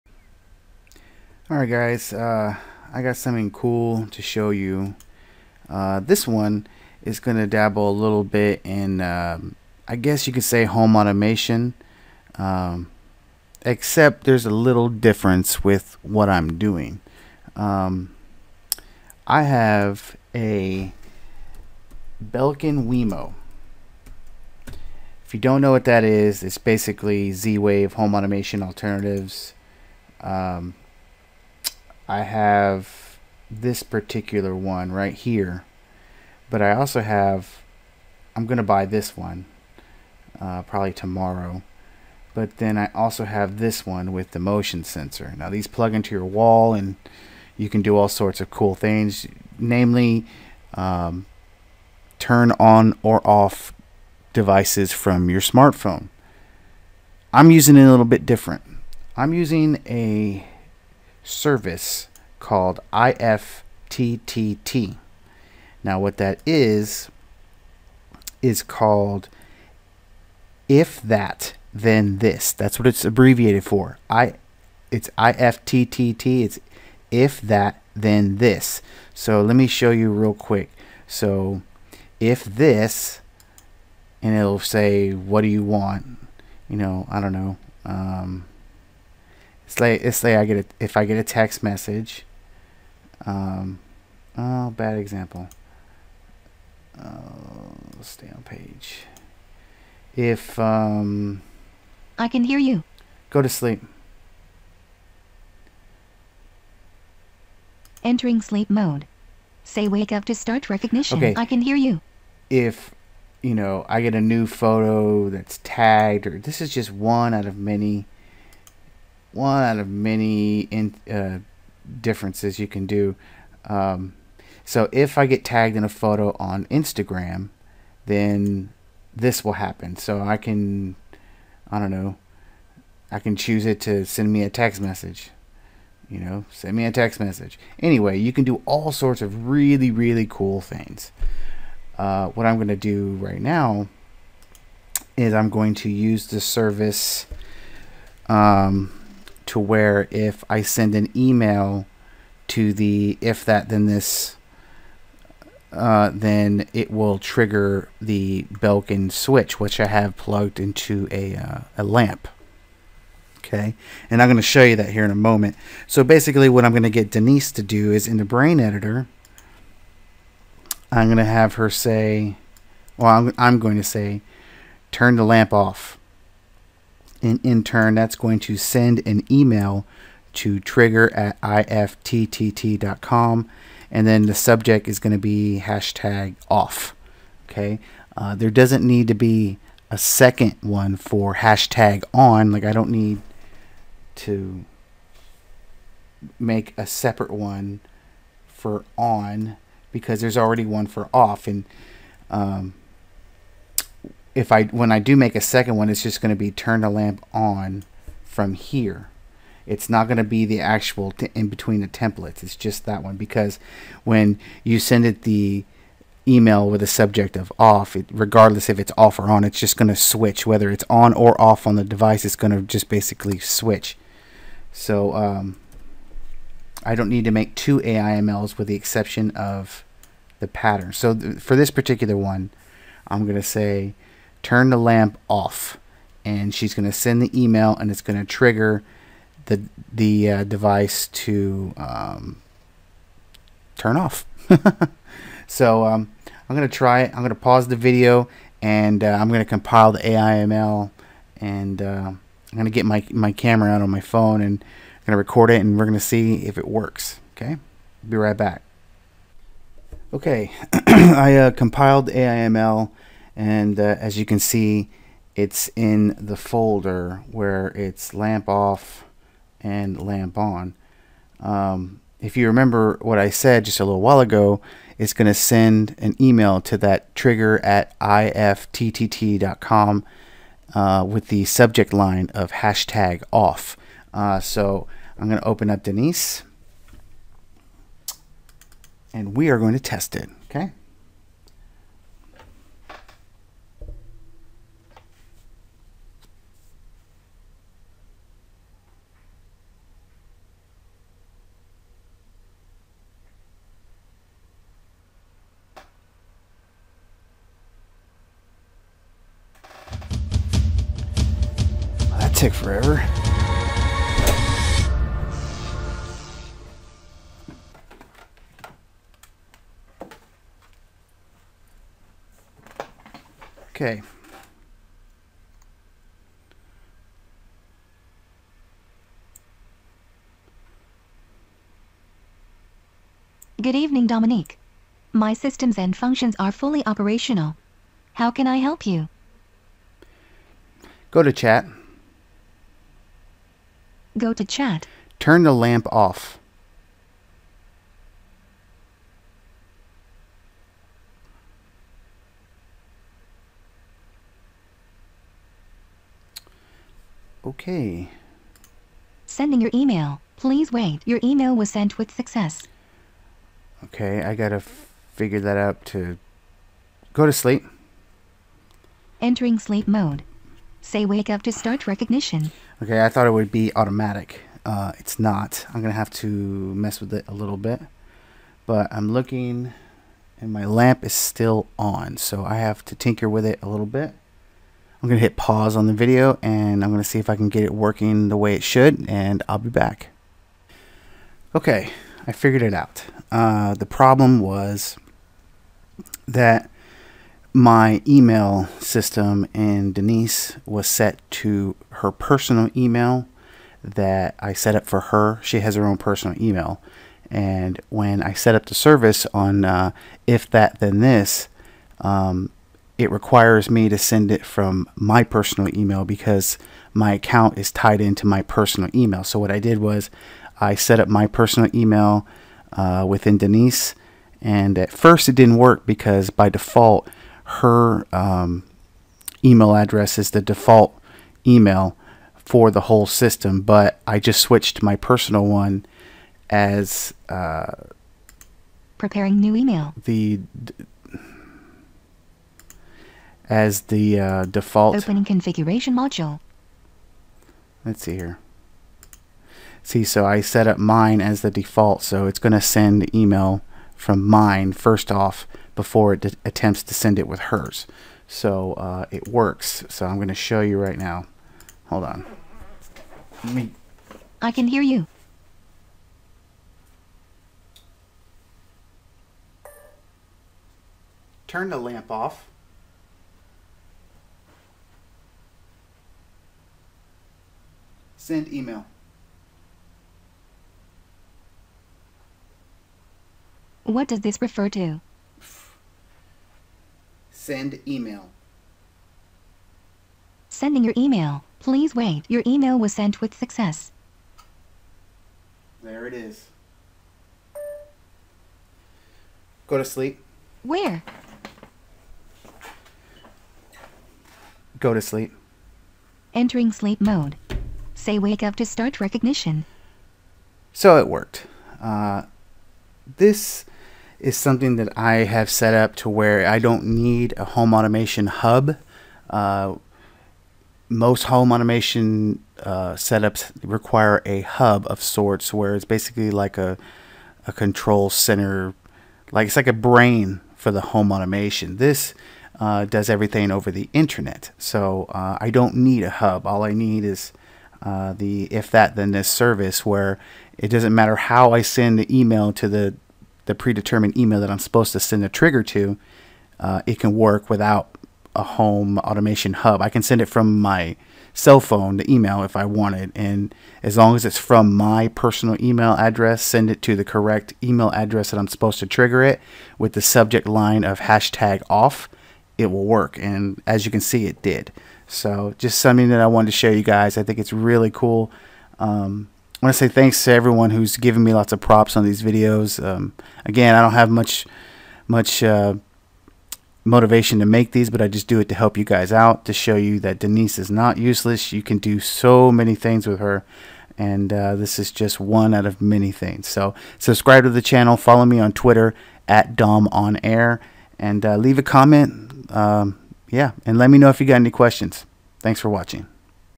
Denise Turning Off A Lamp
In this video I coded Denise to turn off a lamp using a Belkin Wemo and a service called IFTTT.  This was done over the internet and did not require a home automation hub.